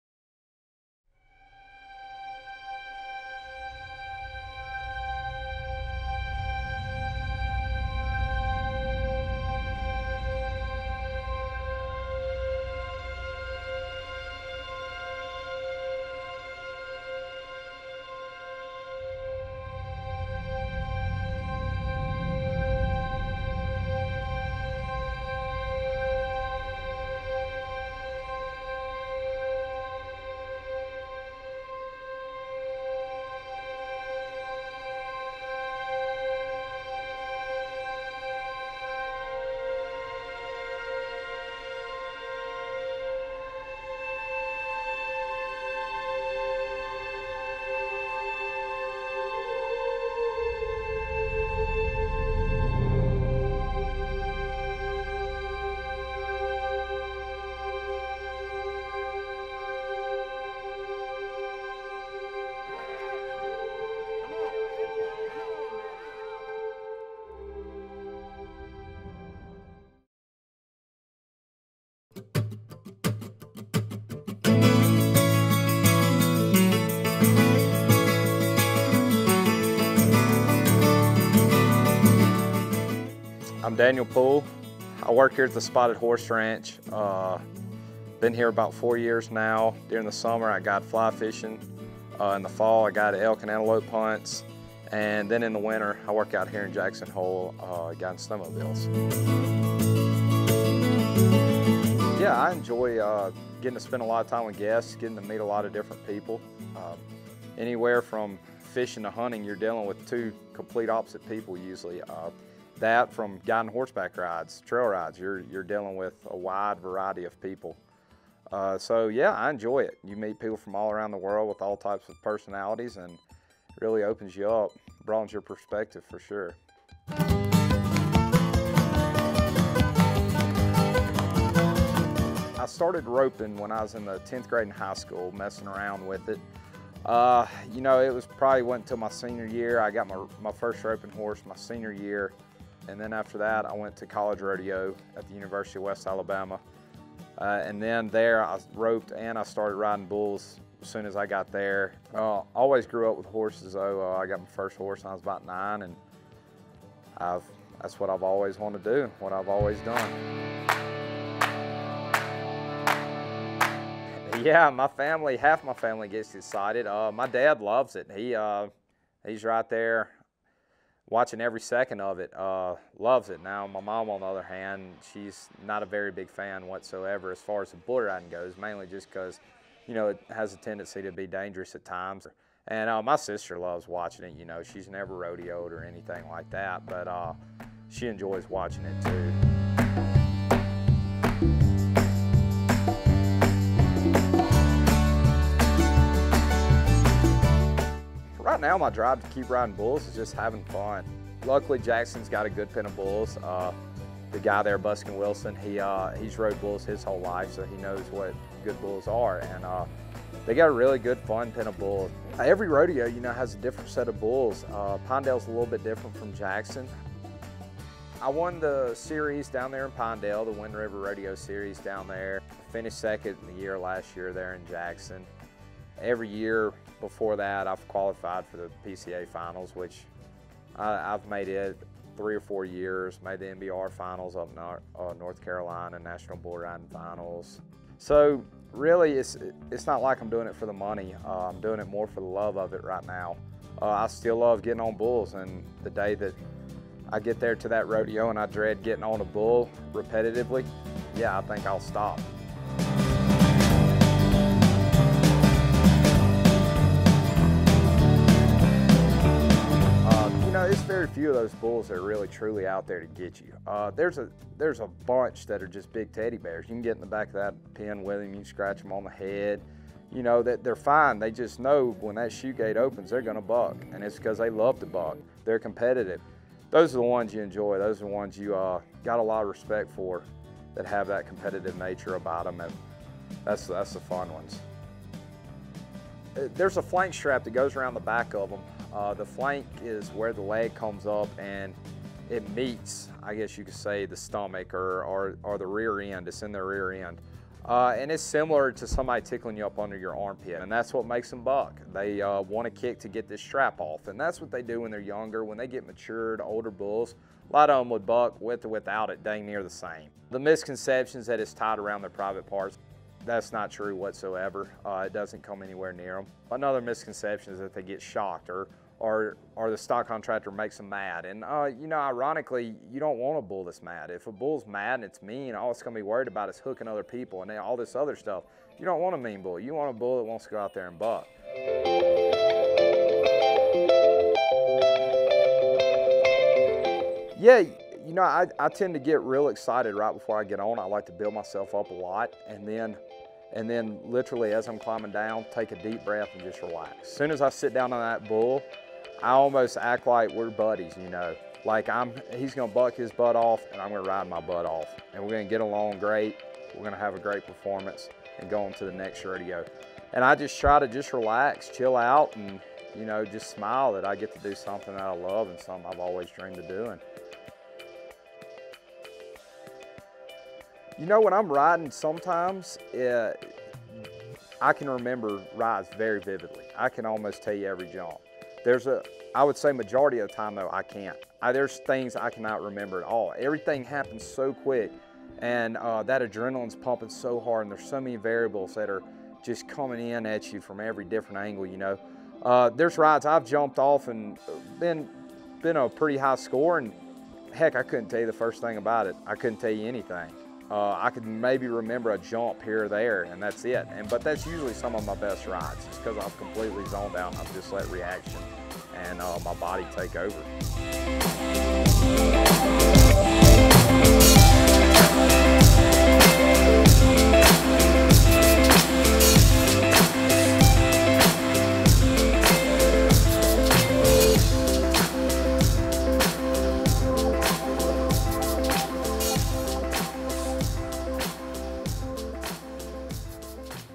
Wyoming Ride
A short documentary on one of our employees Dan Poole who has been with us for 5 years now. This is a quick look into his rodeo life, hunting guide, wrangling, fly fishing and Spotted Horse Ranch time.

Special Thanks:
Bridger-Teton National Forest
Jackson Hole Rodeo
Teton County Fairgrounds
Wade Miller
Wind River Rodeo Series
The Poole Family

Permitee Bridger-Teton National Forest
Equal Opportunity Provider